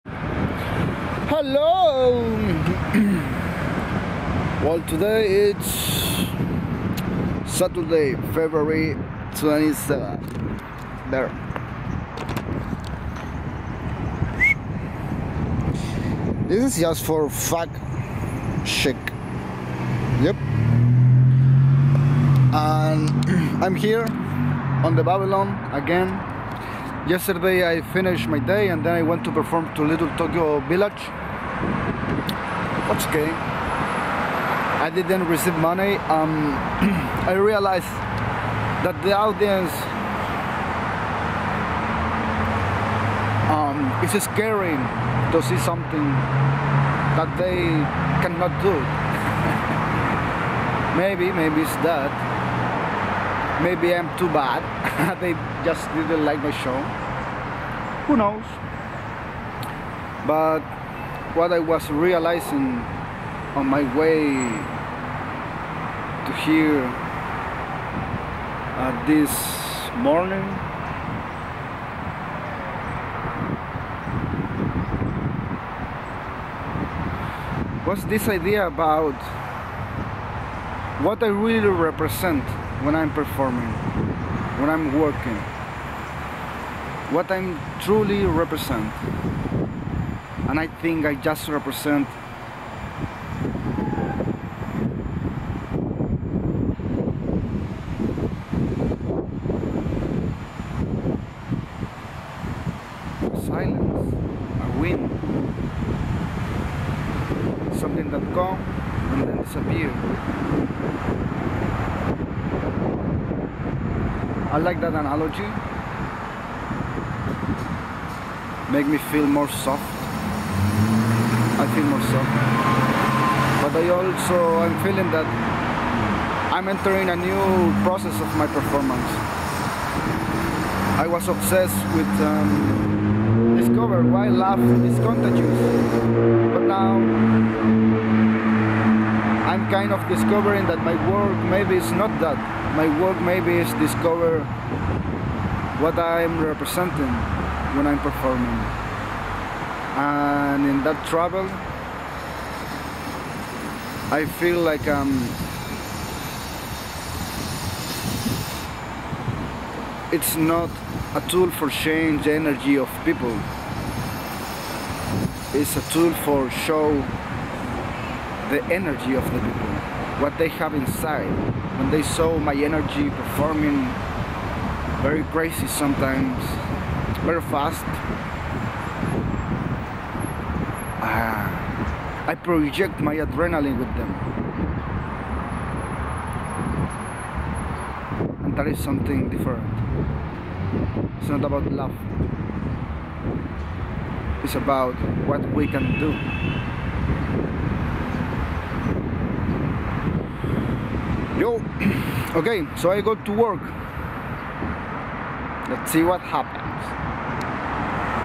DAY 23 - 100 days street performance in Los Angeles, California, 2021.
$upport if you can DAY 23 / FEBRUARY 27

RAW MATERIAL / HONEST DOCUMENTARY

THIS IS A WAY TO EXPLORE PERFORMANCE OUTSIDE THE BIG CORP OF PERFORMING ART, THIS IS A WAY FOR REFUSE TO GET PAID BY CAPITALISM, THIS IS ACTION, REVOLUTION, AND LOVE FOR THE HUMAN CONDITION